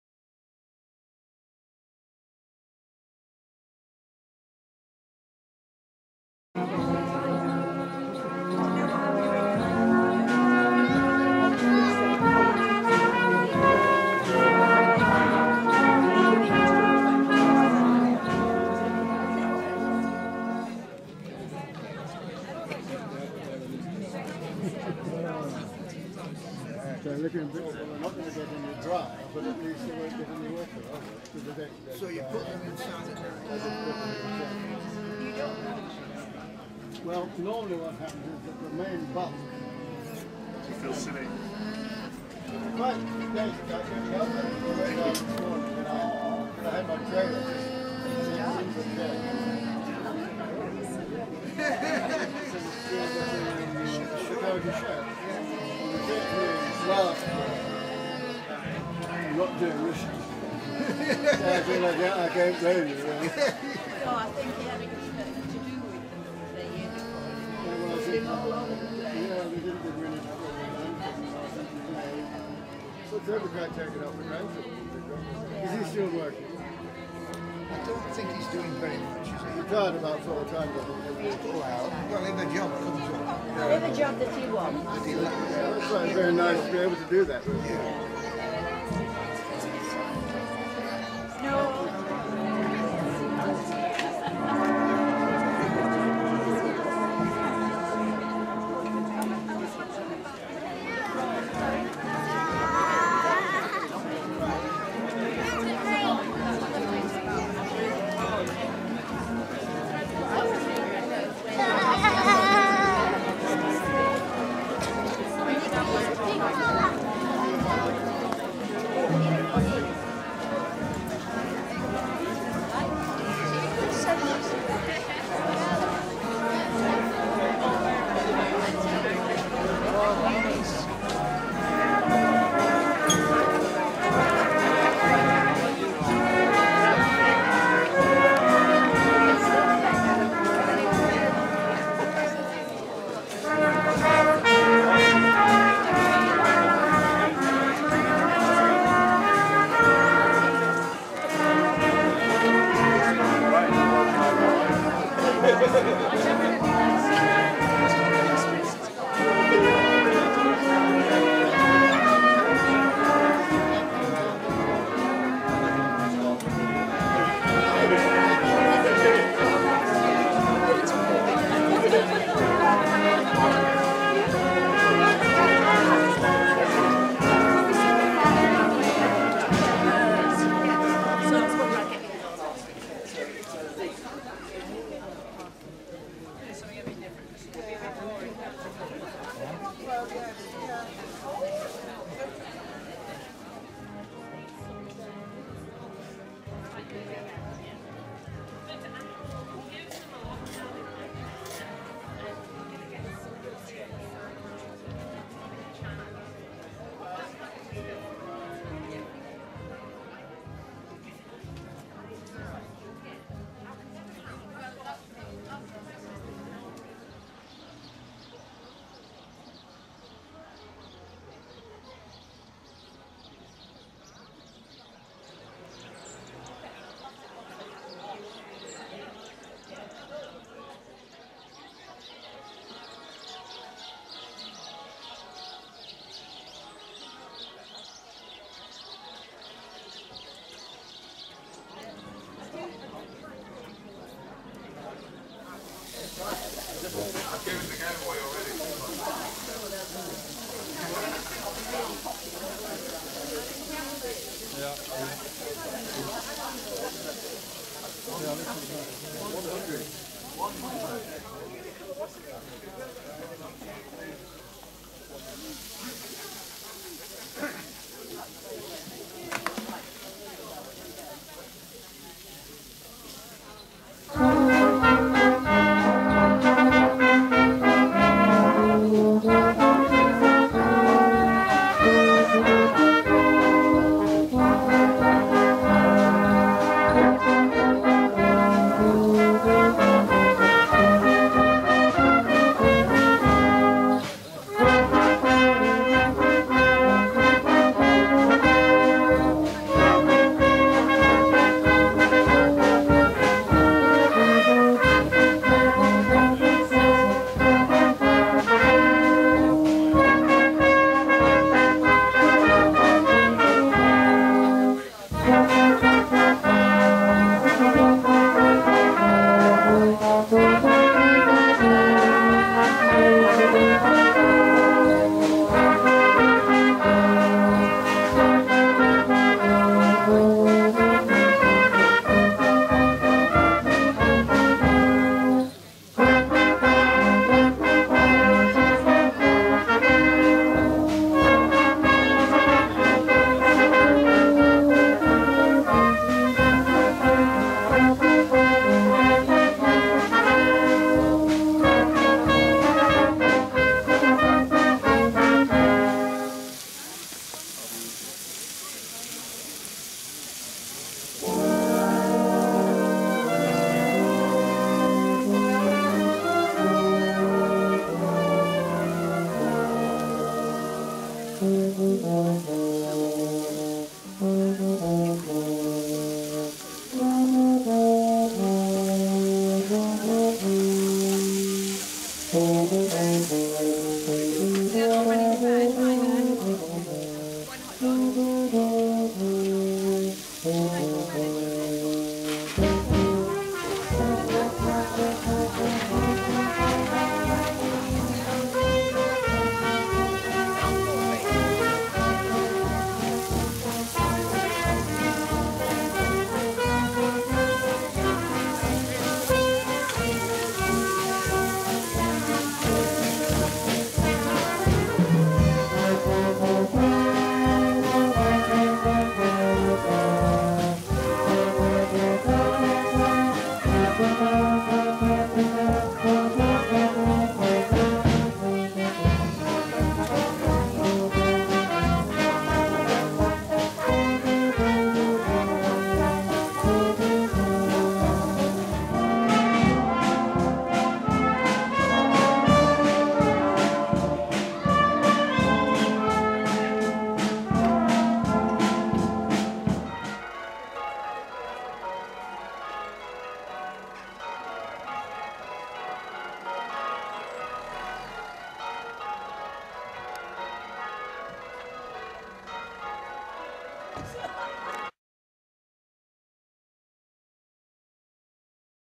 Orton Farmers' Market - Christmas '16. (2)
With a Brass Band and a choir in the Parish church, it was all systems go for the monthly market. Those who play wind instruments will appreciate that being in the open air is not best for maintaining the right note!